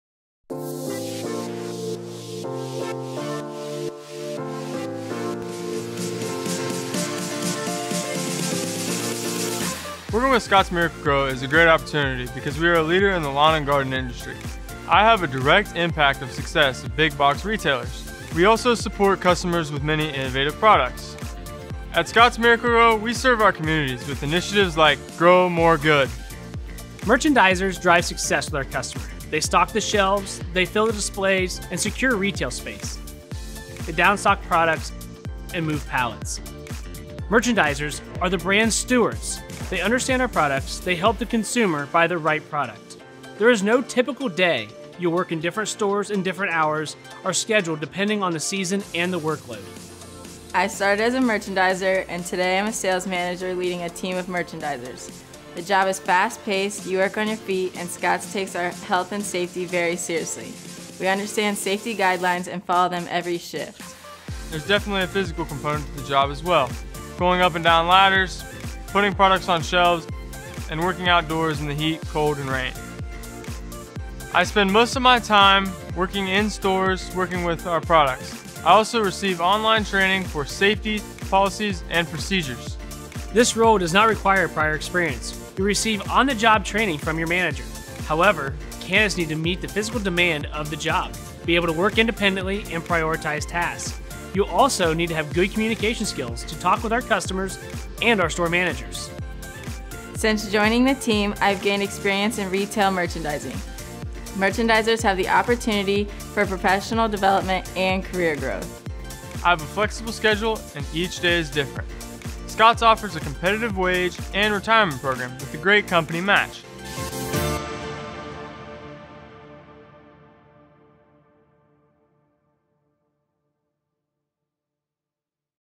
Be a Merchandiser at ScottsMiracle-Gro
Learn about what it's like to be a Merchandiser at The Scotts Miracle-Gro Company.

This isn’t your average seasonal, part-time position. Our merchandiser role provides you with the opportunity to have flexibility, work with leading U.S. retailers and build a career.

You’ll work with and learn about our top lawn and garden brands, allowing you to develop relationship-building skills and sales acumen.

We’re doing big things this year. Join our merchandiser team and be on the frontlines working with retailers and consumers on behalf of our company.